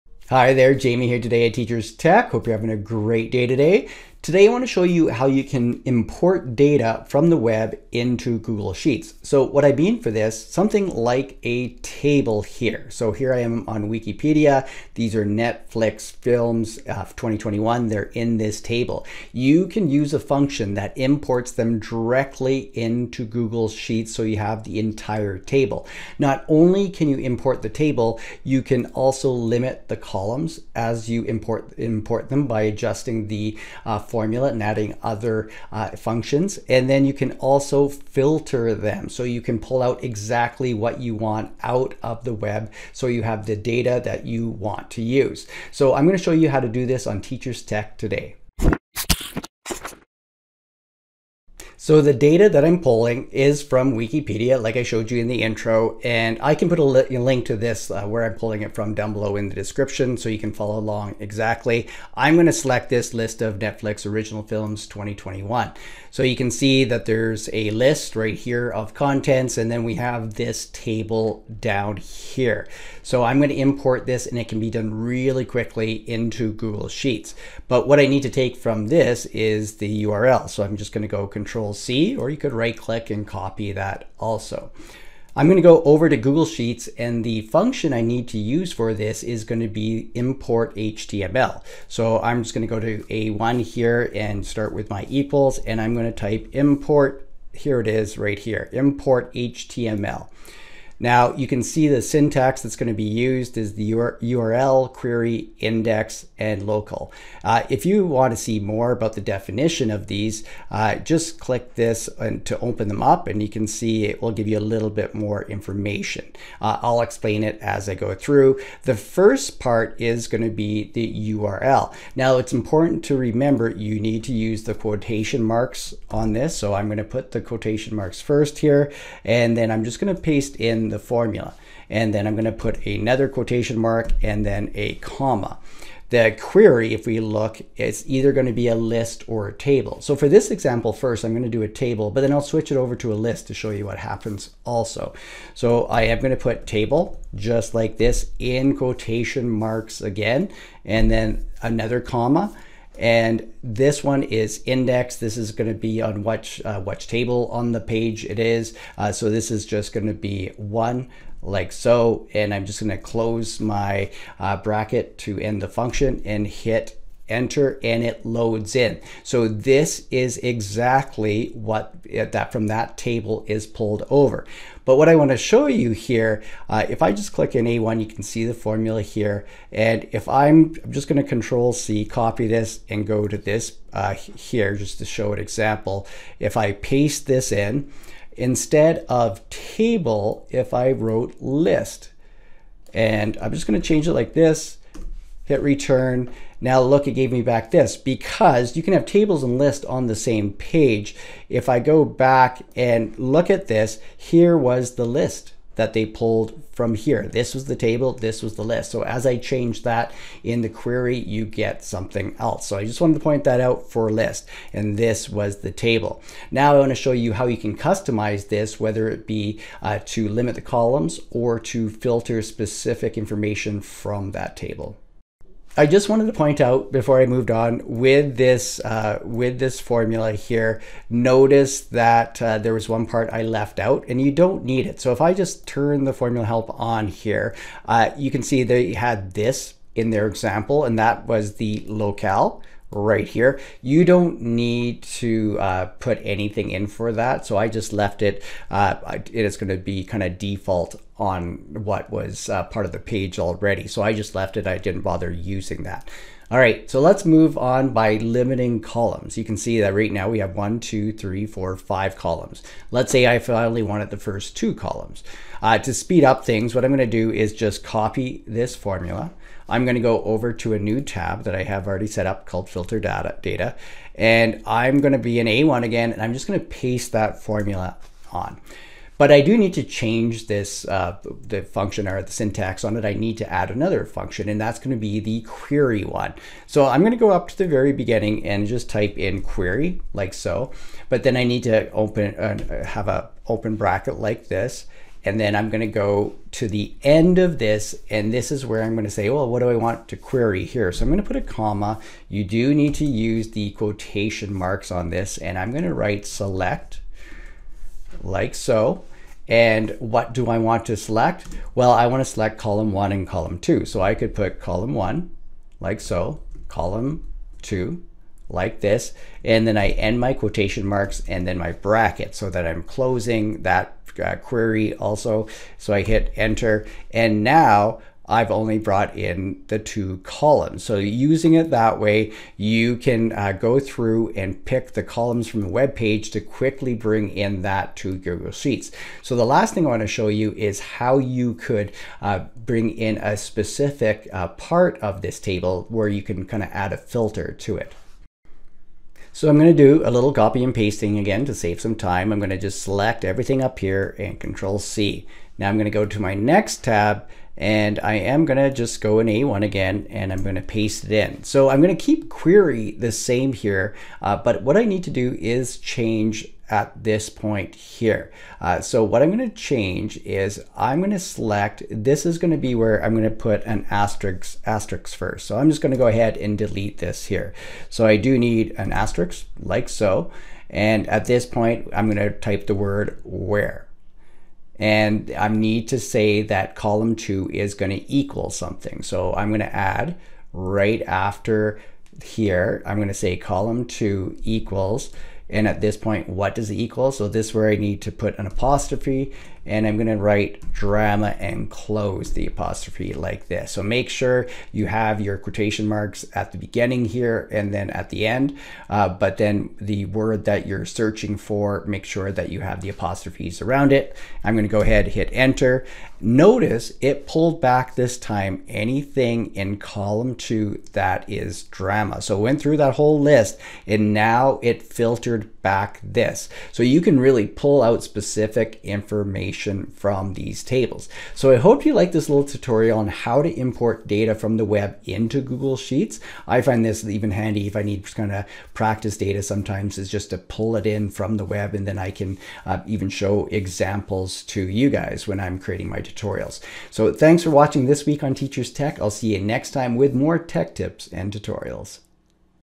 How to Import Data from Webpages into Google Sheets
This video will show you how to import tables and lists from a webpage to Google Sheets. Using the IMPORTHTML function in Google Sheets allows this import to be done very quickly.  You can customize the data you import from the web by limiting columns or adding filters to get exactly what you want in Google Sheets.  This is done by adding the QUERY function along with the IMPORTHTML function in Google Sheets.

0:00 Introduction
0:50 Import table from a webpage into Google Sheets
3:01 Import list from a webpage  into Google Sheets
3:45 How to limit columns from the import table
5:48 Filter data from the import table into Google Sheets